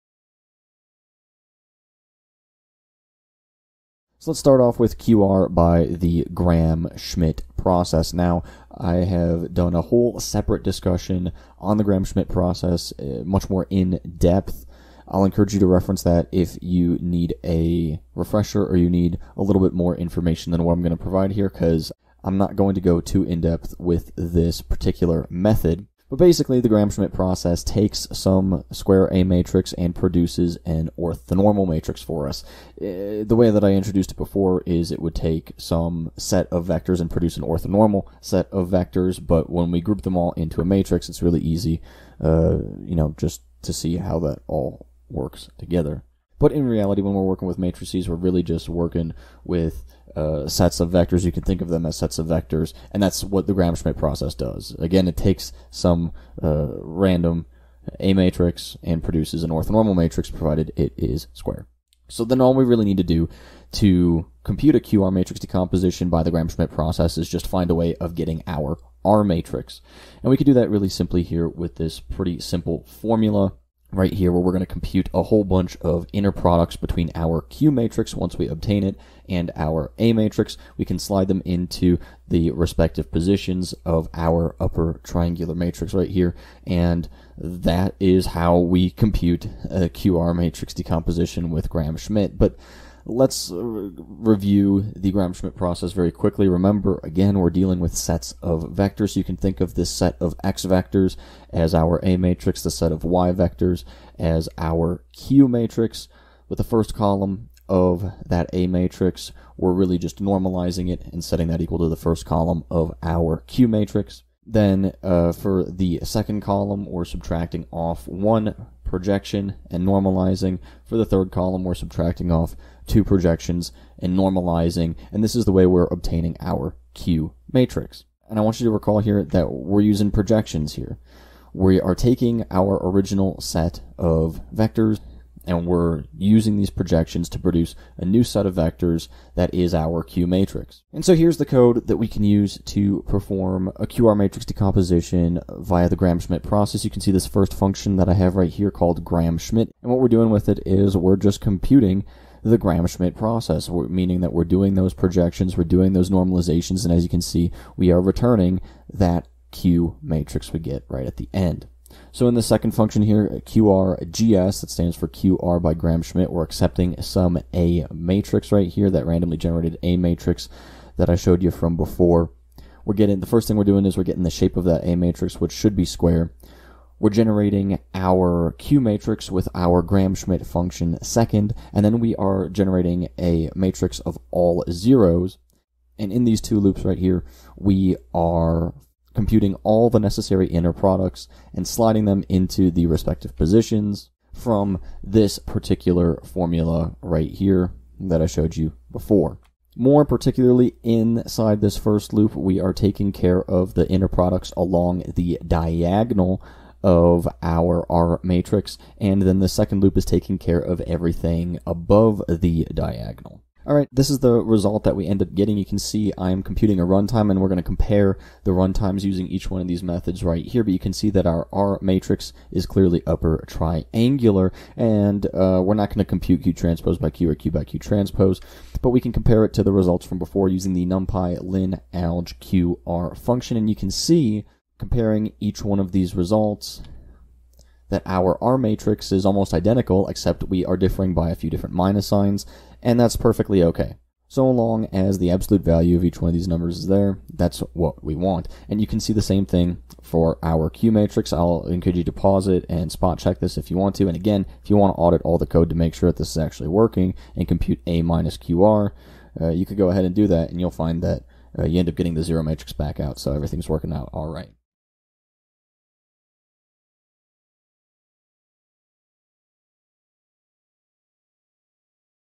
QR Decomposition by The Gram-Schmidt Process - Linear Algebra
In this clip we discuss how to perform a QR decomposition via the Gram-Schmidt process, with example code in python.

This is a clip from a broader discussion on the QR decomposition.